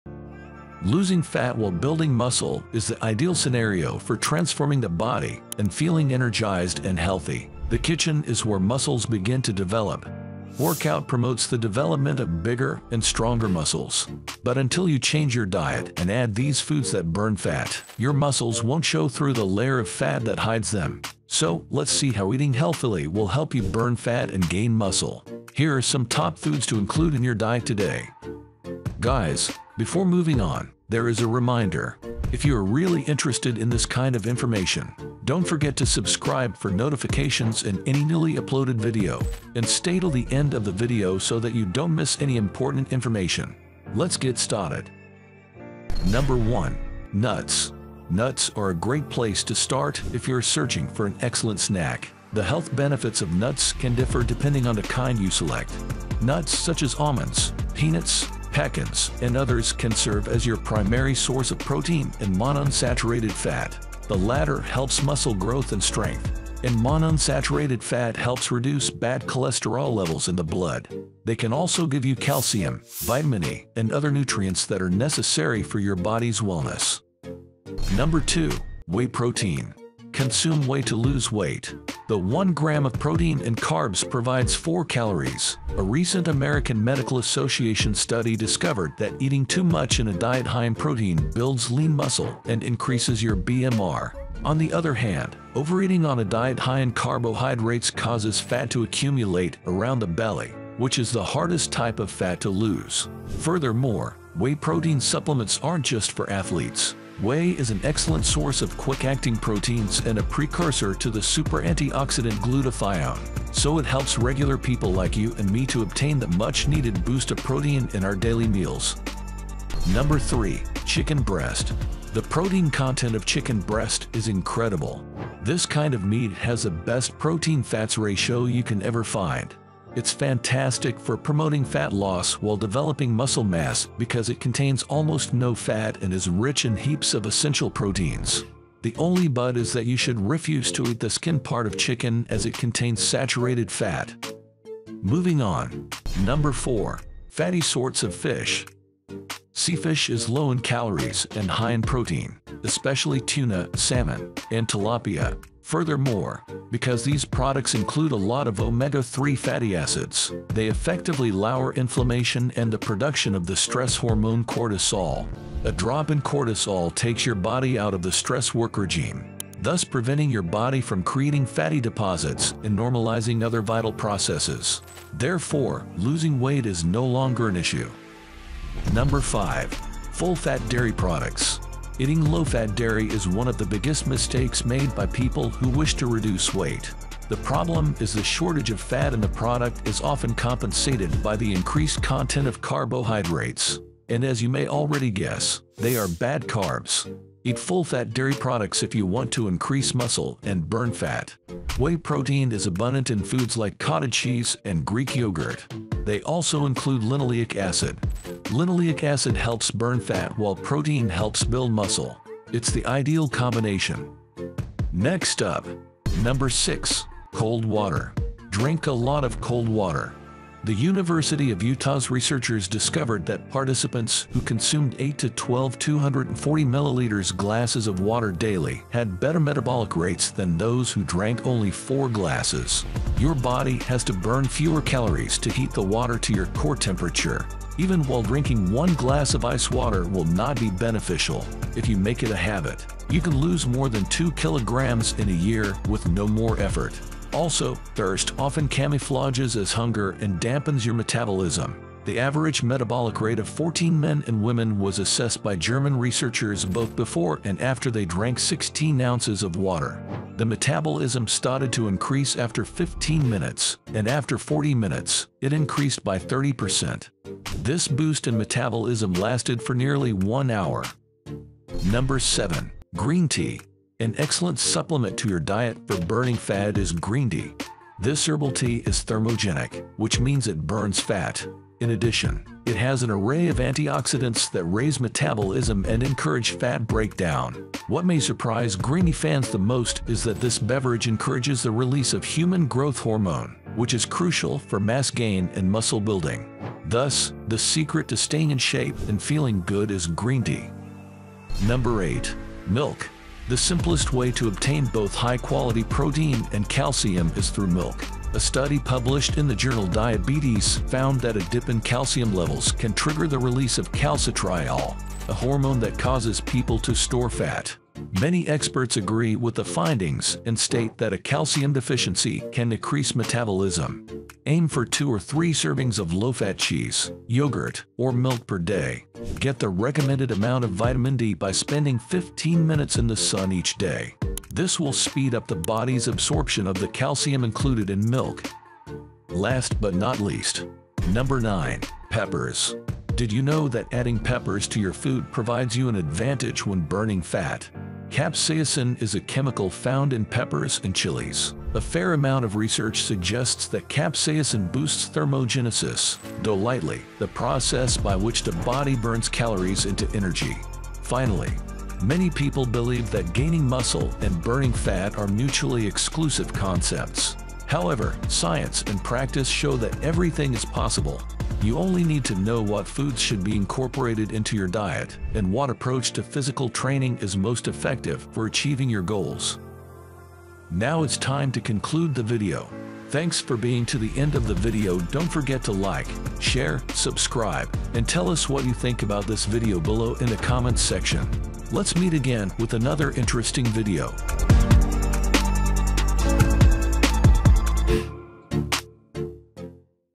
The science of eating for health, fat & lean Muscle
Delve into the fascinating world of nutrition science with our latest video - "The Science of Eating for Health, Fat Loss & Lean Muscle!" This comprehensive guide explores the intricate relationship between food, health, and body composition. Whether you're aiming for fat loss or muscle gain, this video provides evidence-based insights to help you make informed dietary choices. 🥦🔬

💡 Uncover the secrets behind nutrient-rich foods that support overall health, delve into the science of macronutrients for optimal energy balance, and discover the strategic approach to fueling lean muscle growth. From metabolism boosters to muscle-preserving foods, we've got you covered! 🏋️‍♂️💪

Let's demystify the science of eating together and empower ourselves with the knowledge to achieve a healthier, stronger, and more energized lifestyle! 🔄🌟